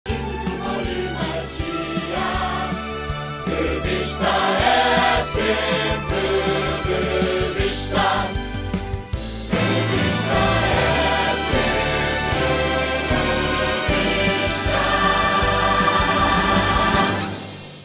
Assista a REVISTA É SEMPRE REVISTA 5ª Feira Dia Nacional dos Bombeiros!
REVISTA É SEMPRE REVISTA é considerado um dos melhores espetáculos de sempre de Filipe La Féria, uma festa de alegria e bom humor onde se reúne os grandes números e estrelas da Revista à Portuguesa e se satiriza, com graça e bom gosto, tudo o que se passa em Portugal!

Um elenco de jovens grandes talentos como Anabela, FF, Paula Sá, Filipa Cardoso, João Frizza, Rafaela Monteiro, Ricardo Raposo, Élia Gonzalez, Paulo Miguel Ferreira, Filipa Azevedo, Jonas Cardoso, Paula Ribas e Sara Cabeleira, um grupo excecional de bailarinos e uma orquestra ao vivo fazem de REVISTA É SEMPRE REVISTA um espetáculo ímpar na história do Teatro Português.

REVISTA É SEMPRE REVISTA
De 4ª feira a Sábado às 21h e Sábado e Domingo às 17h

Reserve já o seu lugar através dos números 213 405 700 ou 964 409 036 ou